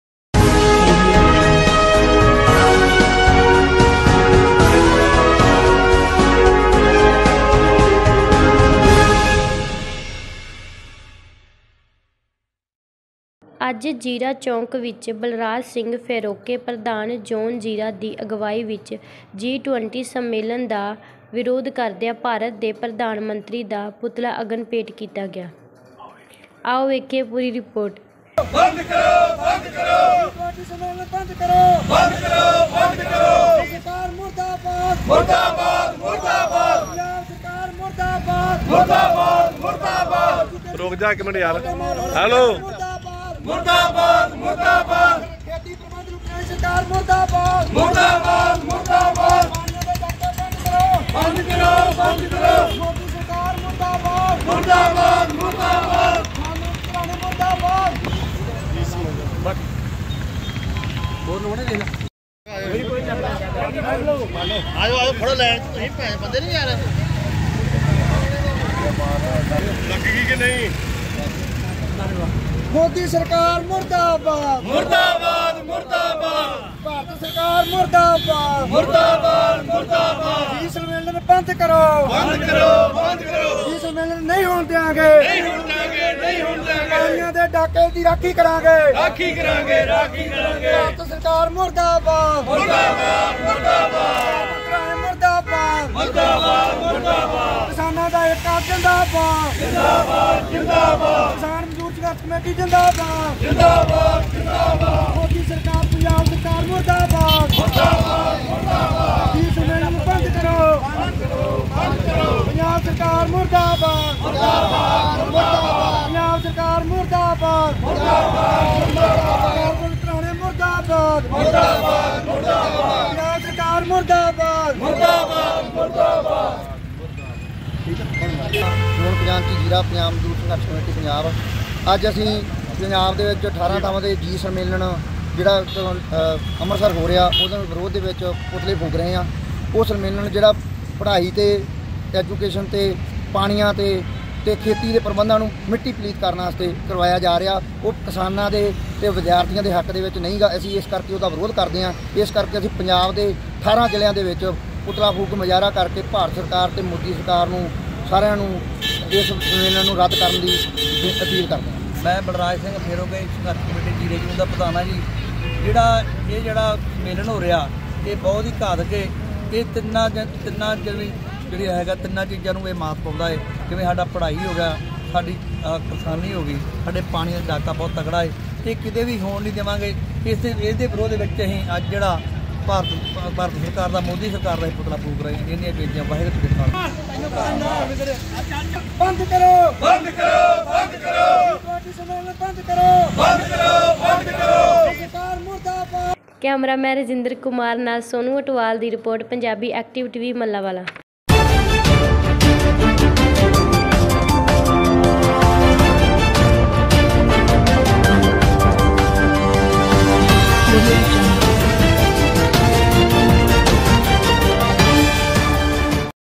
JEERA CHOK CH MODI SARKAR DA PUTLA AGAN BHET KITA GIAA --PUNJABI ACTIVE TV
JEERA CHOK CH MODI SARKAR DA PUTLA AGAN BHET KITA GIAA